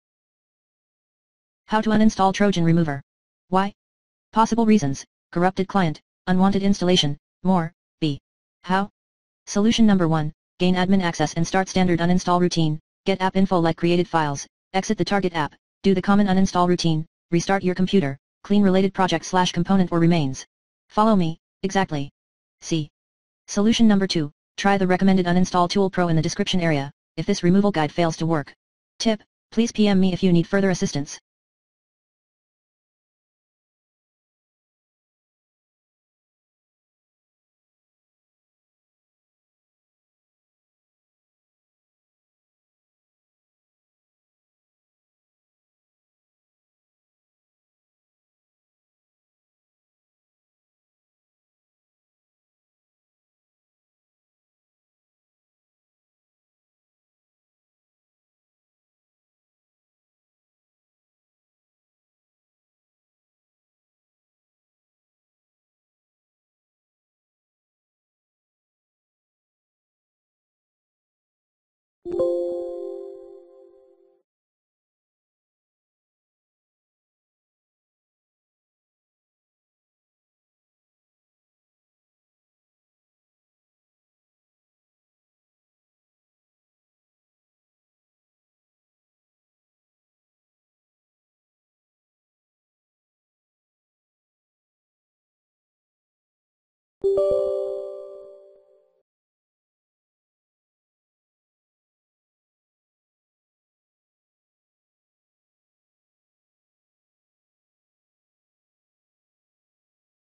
How to Uninstall Trojan Remover 6 on Windows 10?
TRY this best uninstall tool@ if U R having issue uninstalling Trojan Remover and/or any other unwanted programs on Windows.